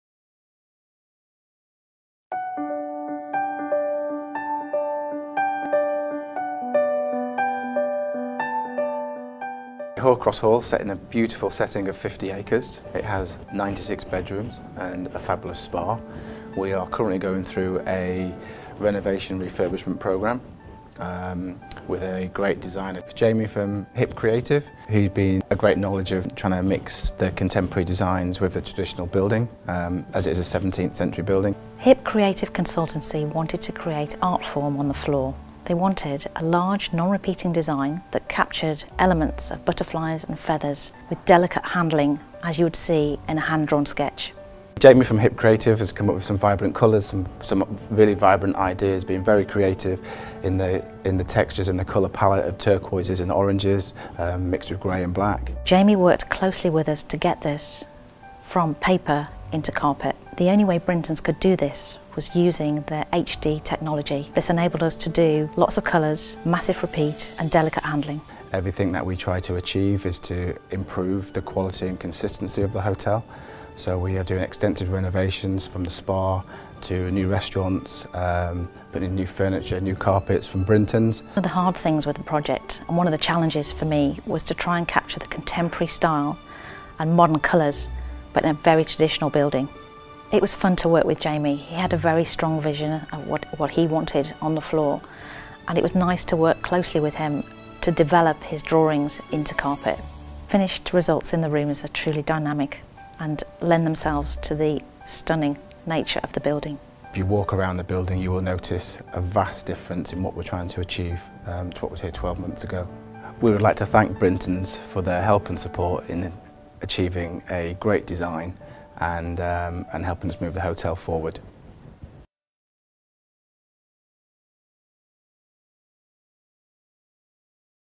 Brintons case study Hoar Cross Hall, Staffordshire
Brintons- მა Hip Creative Consultancy-თან პარტნიორობით შექმნა შესანიშნავი ხალიჩა, რადგან გადაეკეთებინათ ძვირადღირებული კურორტის Hoar Cross Hall- ის ულამაზესი სამეჯლისო დარბაზი, ბიბლიოთეკა, Meynell Suite და დერეფნები.